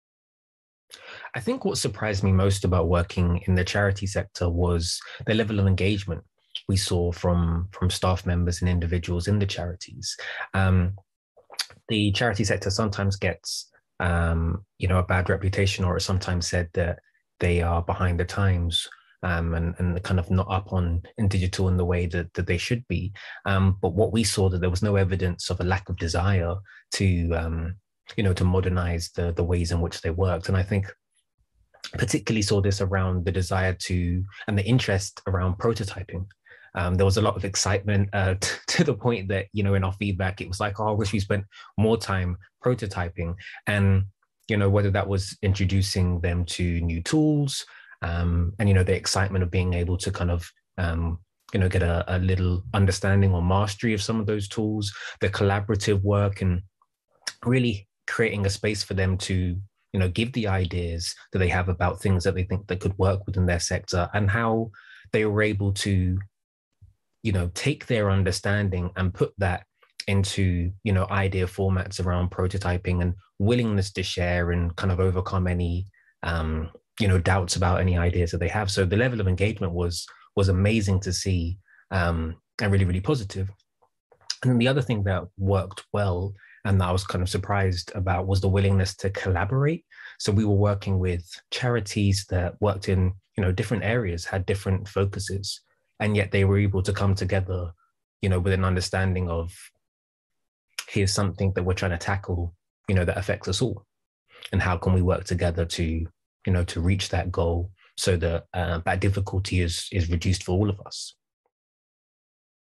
Working with charities: Joshua McGovern on some surprising discoveries
Customer research company Tectonic entered the charity space earlier this year, and in a new series of videos, they share insights on the barriers, the benefits - and the best ways of working. In this video, the company's co-founder & Head of Research Joshua McGovern shares what surprised him as the company moved into the charity space for the first time.